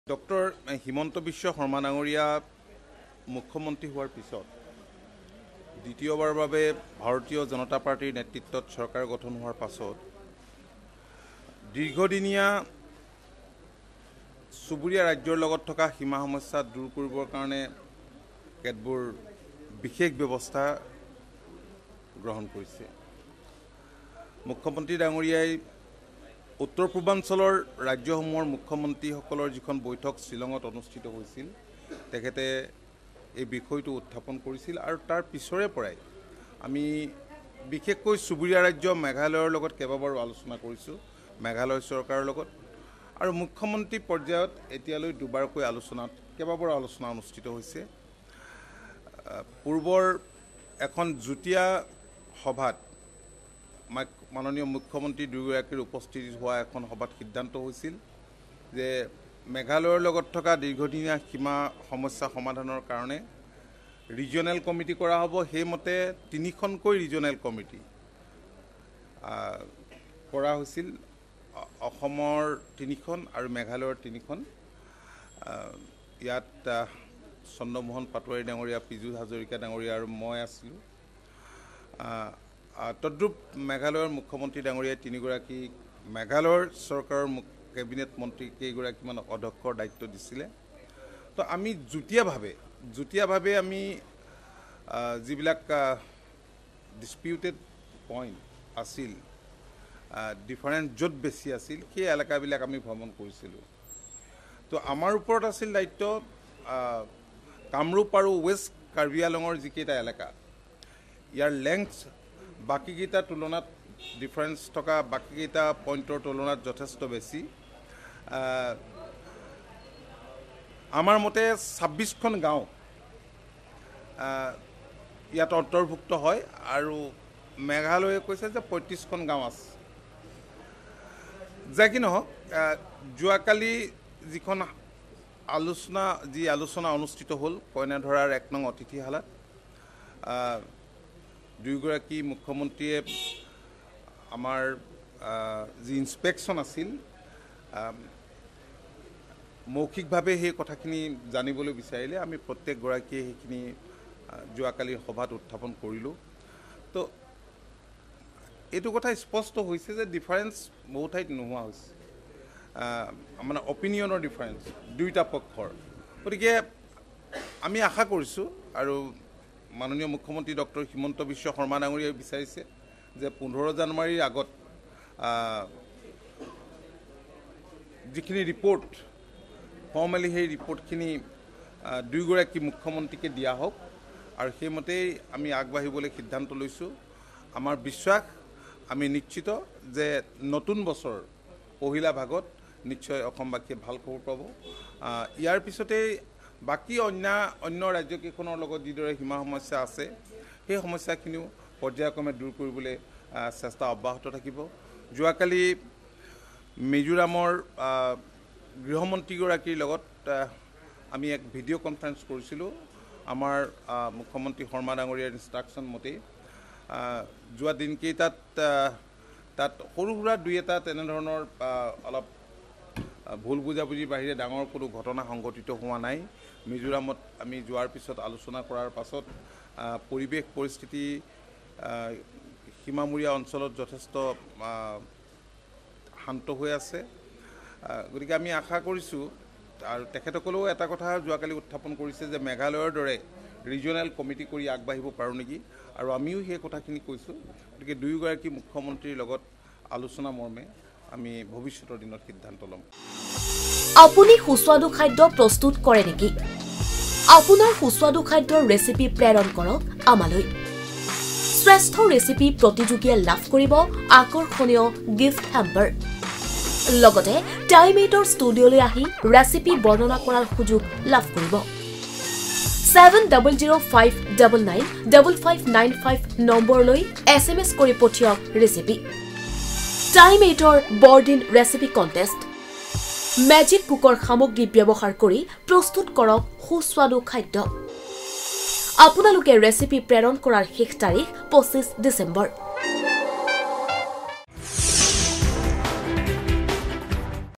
'নতুন বছৰৰ পহিলা ভাগতে অসমবাসীয়ে সীমা সমস্যাক লৈ ভাল খবৰ পাব'
'নতুন বছৰৰ পহিলা ভাগতে অসমবাসীয়ে সীমা সমস্যাক লৈ ভাল খবৰ পাব'- অতুল বৰা।